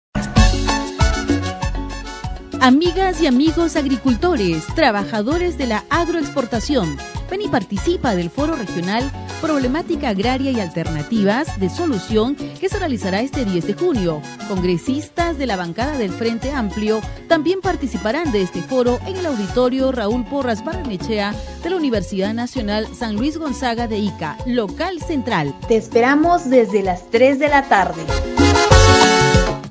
Foro Regional Problemática Agraria, Perspectivas de solución, 10 de Junio en Ica.
Foro Regional Problemática Agraria, Perspectivas de solución, 10 de Junio en Ica, Auditorio Javier Porras Barnechea, invitación a  agricultores y población en general.